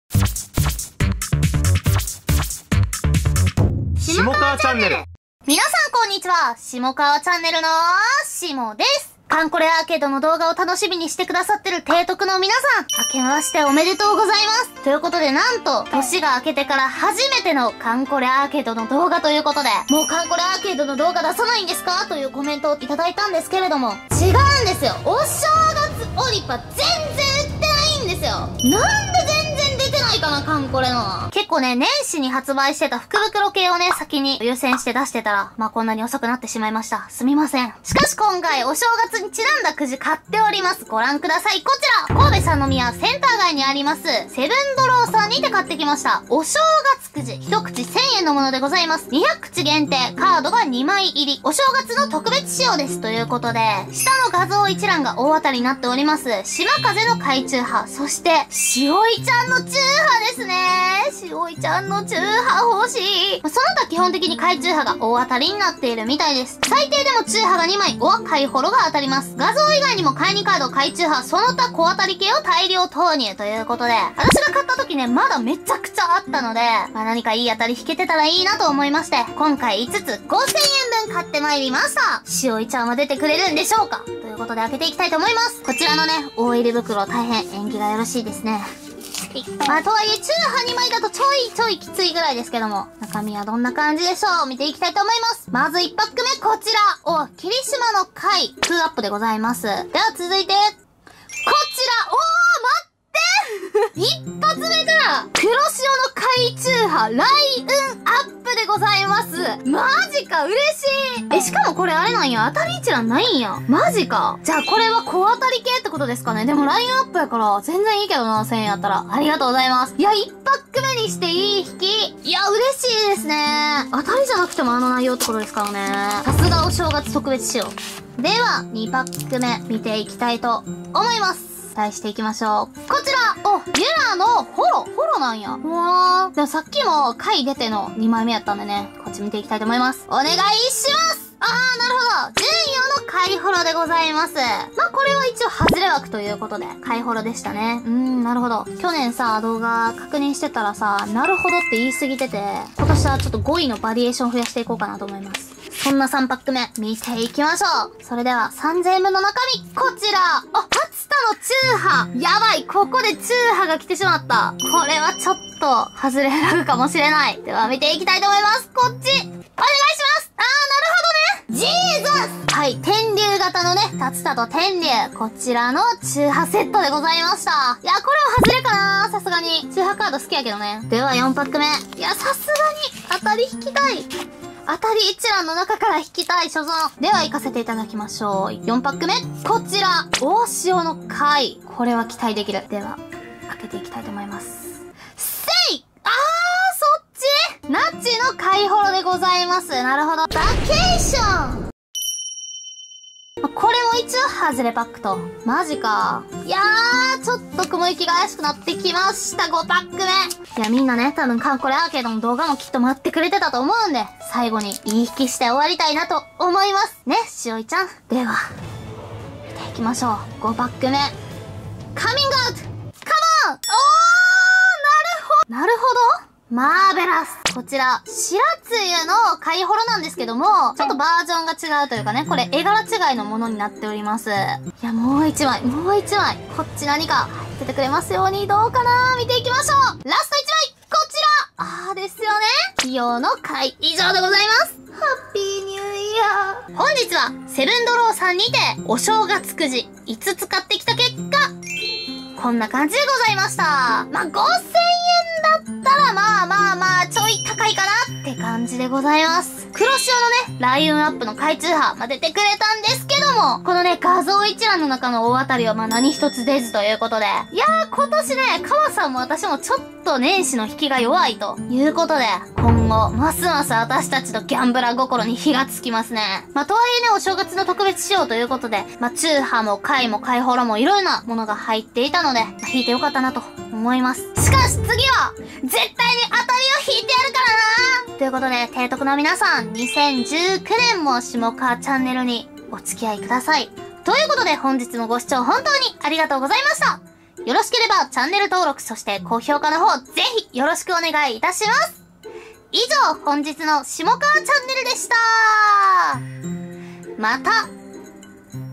【艦これ】「本音を話します。」沈黙を破り、驚きの告白・・・【開封動画】
提督の皆さん・・・

あけおめええええええええええええええええええええええええ！！！！！！！！！

艦これアーケード
お正月1000円くじ５つ買ってきました！
(兵庫・セブンドローさん)

艦これ担当はシモです。

アカウント名：シモカワチャンネル